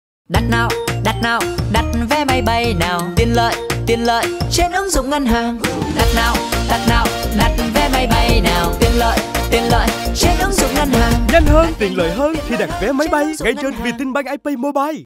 Đặt vé máy bay cực nhanh trên ứng dụng ngân hàng số VietinBank iPay Mobile
Trời xanh, mây trắng, nắng vàng
Muốn bay giá tốt, nhanh tiện có iPay
✈️ Với ứng dụng ngân hàng số VietinBank iPay Mobile, bạn tha hồ bay tới những vùng đất mới, thỏa đam mê xê dịch khám phá khắp mọi miền.
Đặt vé bay ngay, vi vu khắp chốn!

Dành thời gian để tận hưởng cuộc sống, việc khó đã có VietinBank iPay Mobile lo! 💞